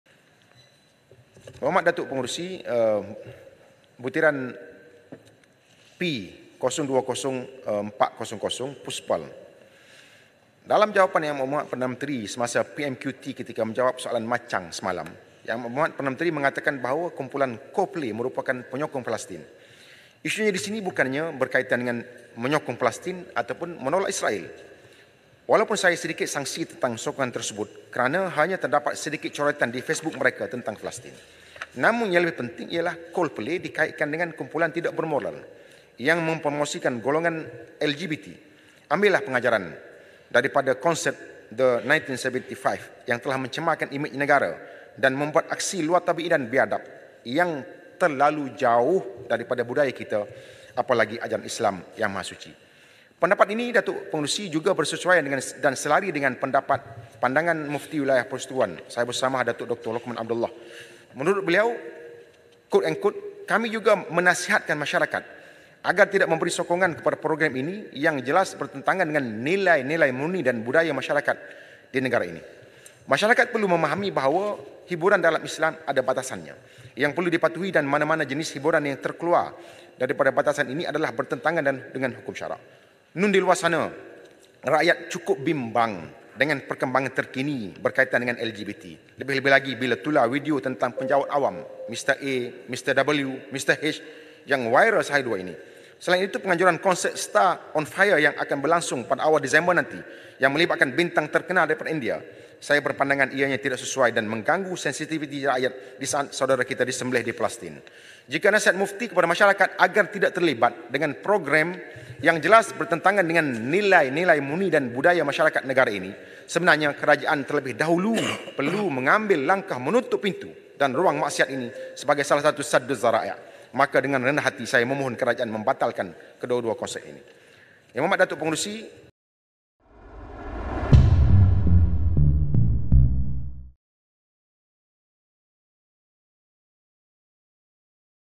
Selepas Coldplay, MP PAS gesa batal pula konsert artis Bollywood
Seorang ahli parlimen PAS hari ini menggesa kerajaan membatalkan konsert Bollywood yang dijadualkan pada 1 Dis ini.

Abdul Latiff Abdul Rahman (PN-Kuala Krai) berkata konsert Stars On Fire yang menampilkan artis terkenal dari India adalah tidak sesuai dan tidak sensitif, terutama ketika rakyat Palestin sedang dibunuh oleh tentera Israel.

Dia juga berkata kerajaan juga perlu membatalkan konsert Coldplay yang dijadualkan di Bukit Jalil malam ini.